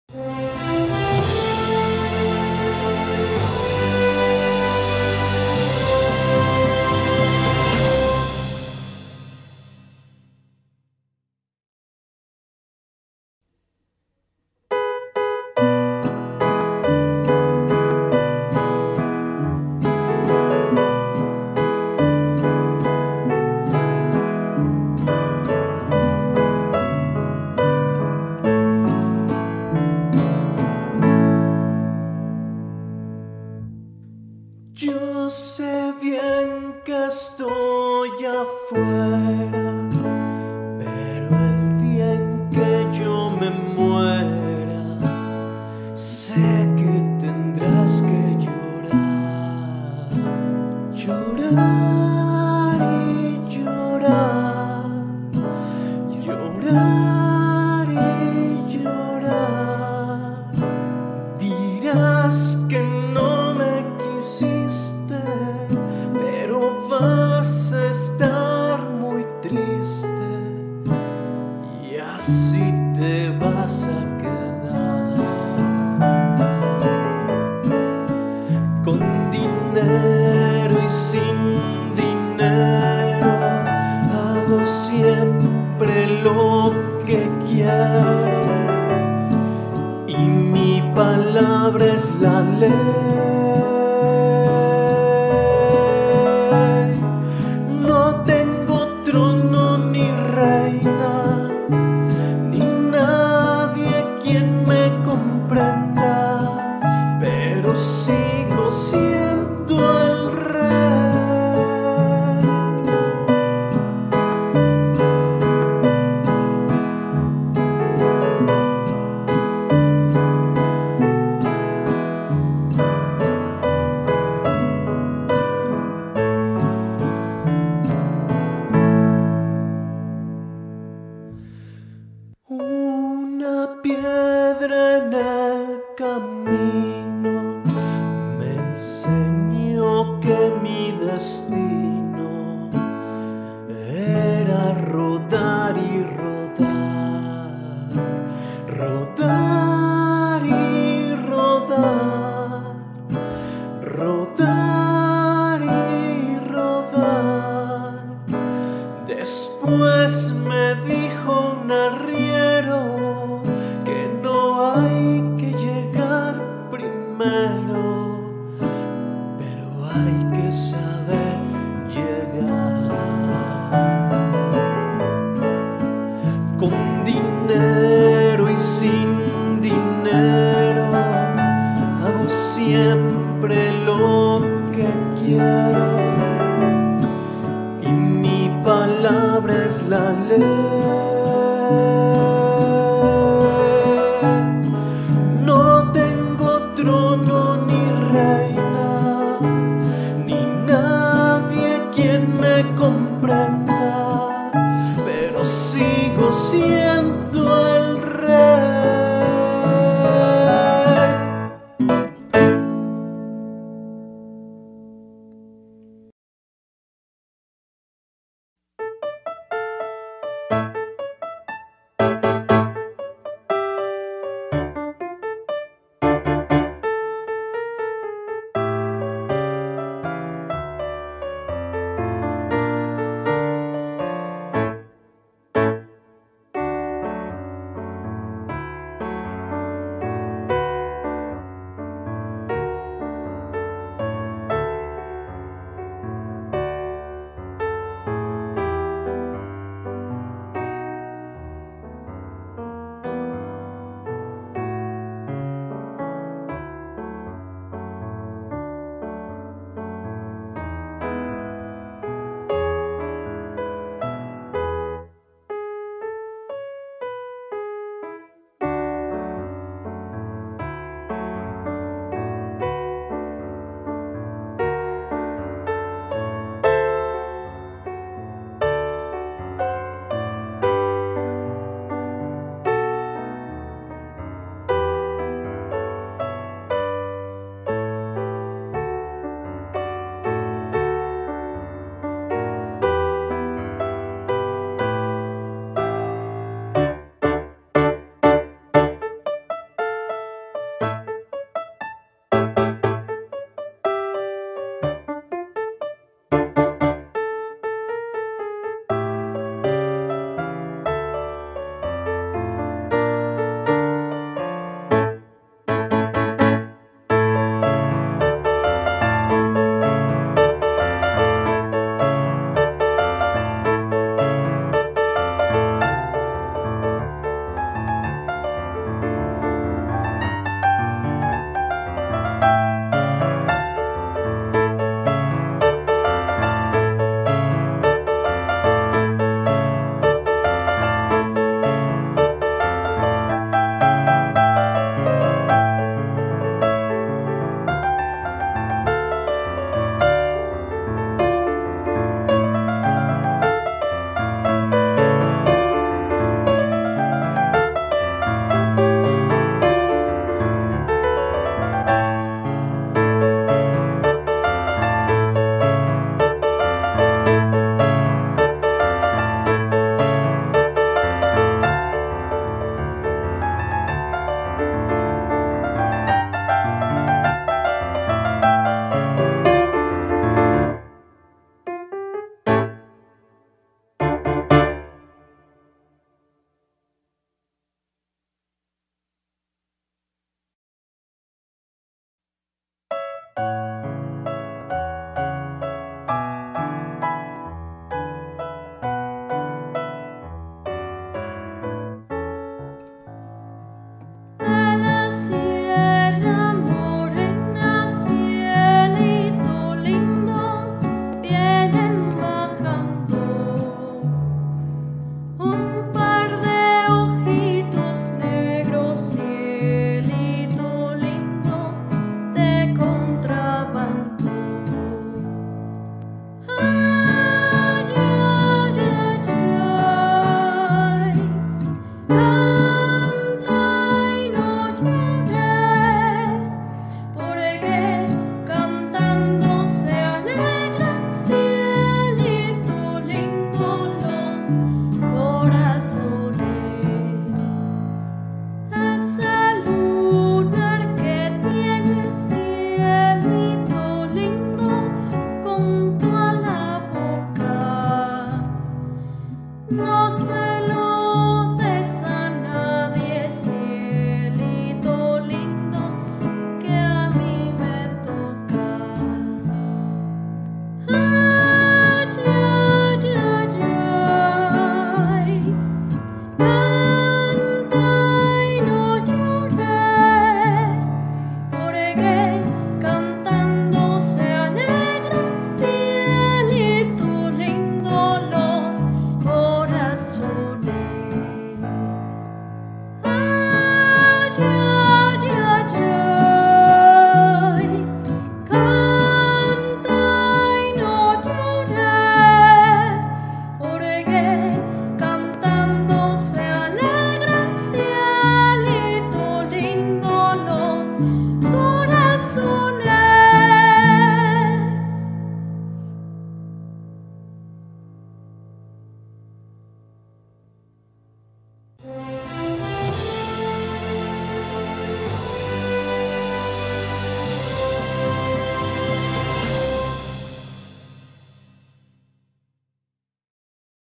Festival Musical Patrio
Participación de docentes de la ESTi:
L.E.L.I. Flor Judith Muñoz Pérez (Voz)
L.C.E. Javier Fuentes Hernández (Voz y Guitarra)
L.M. Oscar Eniel García Cruz (Piano)
(No contamos con derechos de autor sobre la música y letra de las canciones)